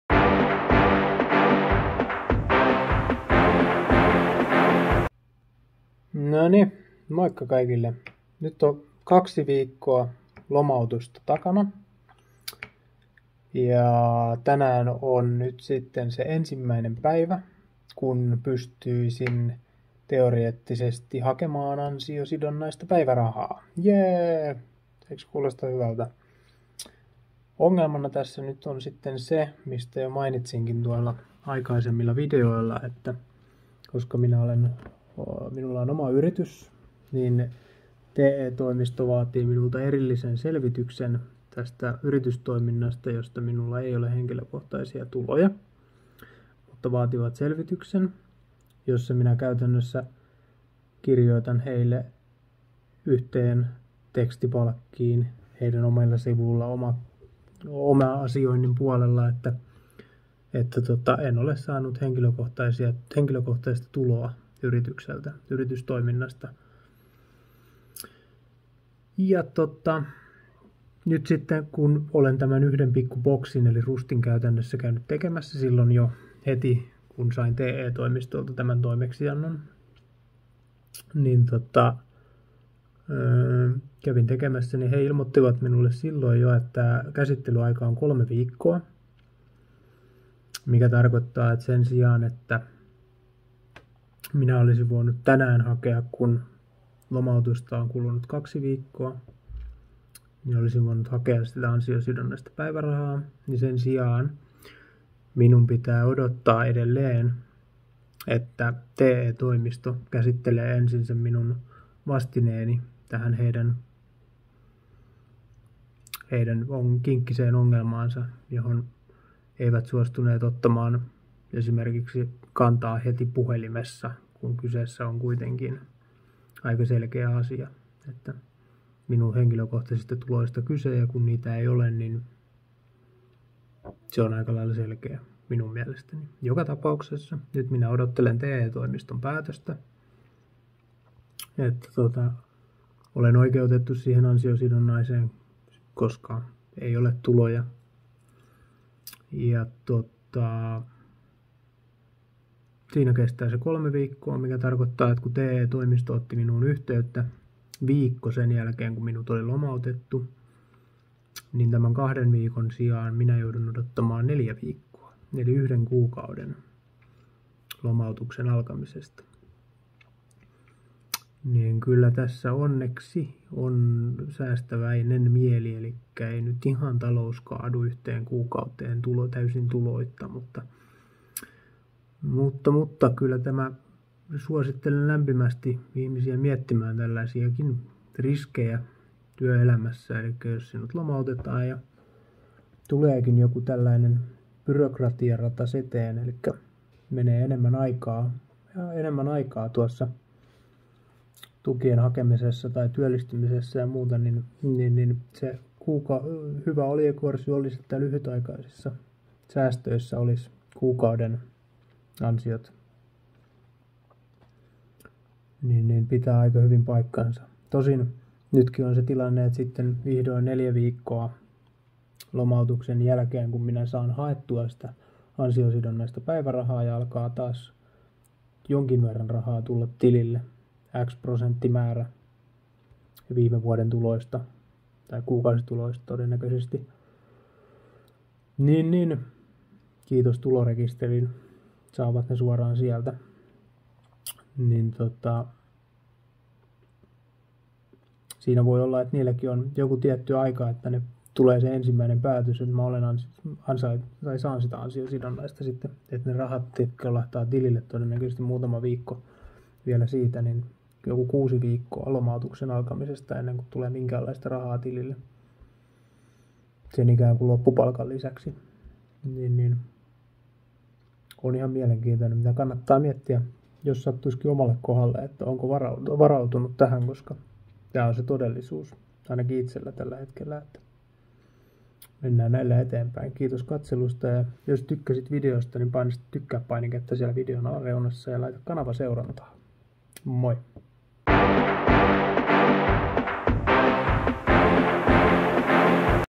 VLOG: Lomautus OSA 2. Mitä tehdä kun lomautetaan? Miten toimia kun lomautus?
Tässä omakohtaisella kokemuksella varustettu lyhytkertomus lomatuksesta ja toiminnasta kun lomautuksesta on kulunut 2 viikkoa.
Muista tykätä videosta ja laittaa kanava seurantaan. ;-)